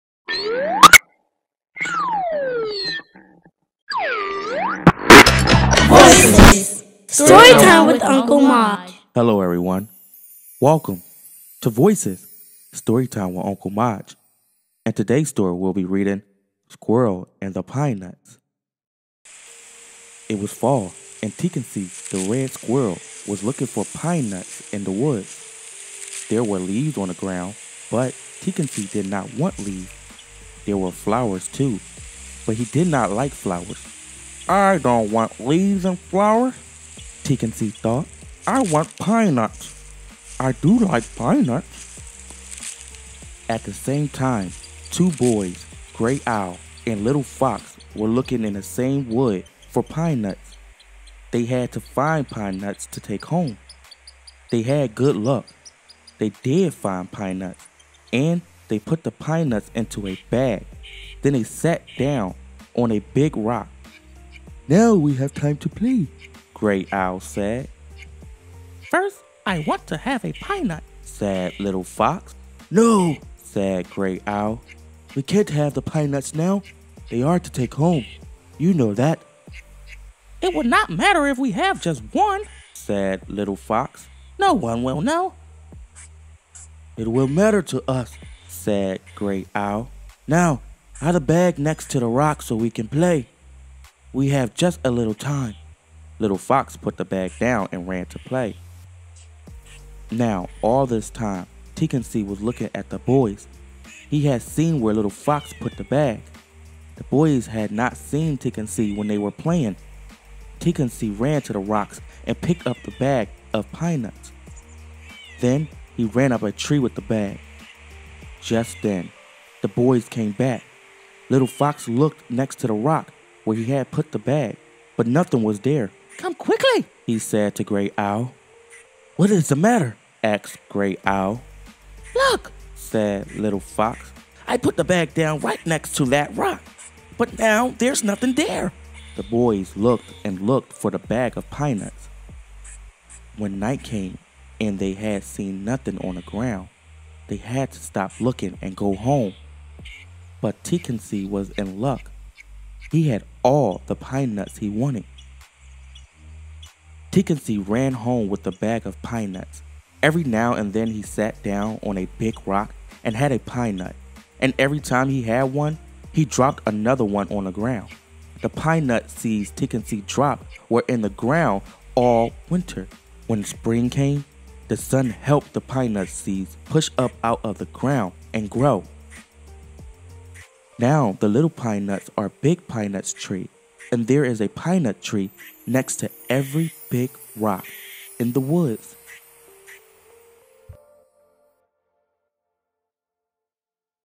Squirrel And The Pine Nuts| Voices StoryTime With Uncle Maj| Children’s Book
Hello Everyone, welcome to Voices StoryTime With Uncle Maj. I hope you enjoy our reading session today! Godbless, come back again!

Retold by: Margaret H. Lippert
Narration and Voiceover by: Semaj Cottle